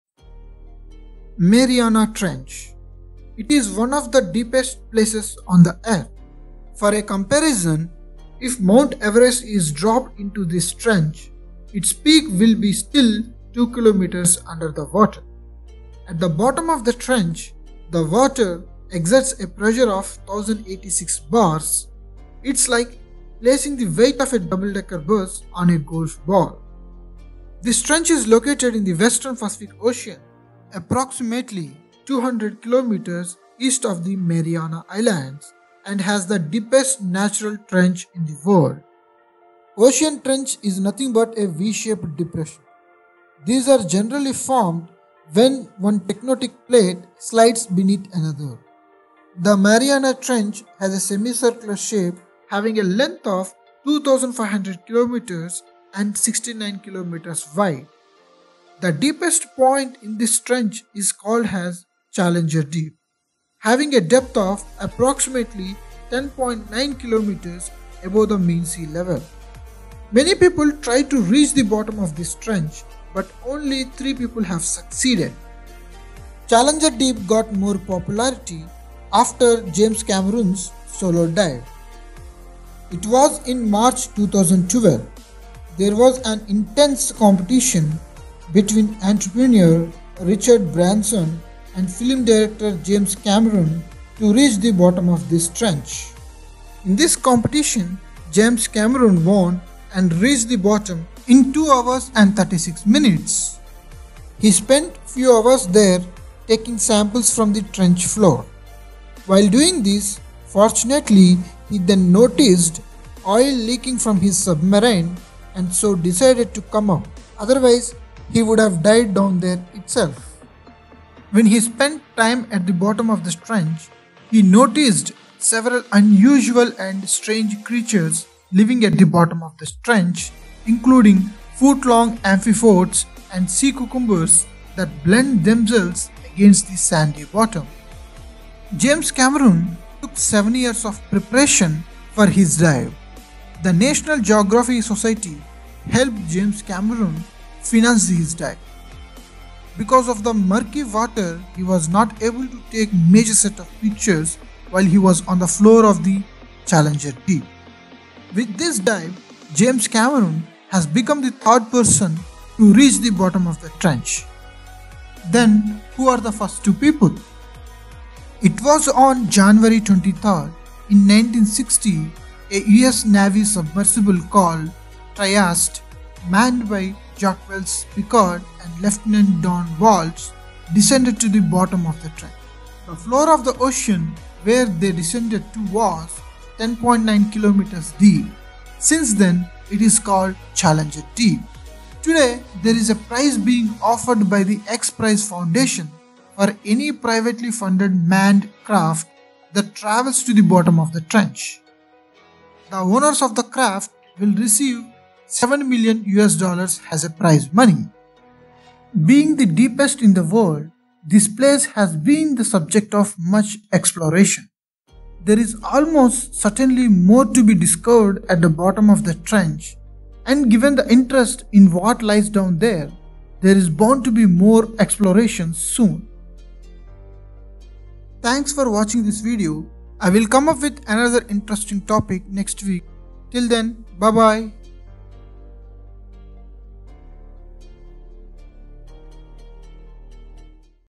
Mariana Trench | Most dangerous and Deepest place on the earth | Challenger Deep
Mariana trench - It is one of the deepest places on the earth.

For a comparison: if Mount Everest is dropped into this trench, its peak will be still two kilometres under the water.

At the bottom of the trench, the water exerts a pressure of 1,086 bars. It is like - placing the weight of a double-decker bus - on a golf ball.

This trench is located in the western Pacific Ocean - approximately 200 kilometers east of the Mariana Islands, and has the deepest natural trench in the world.

Watch video for more updates...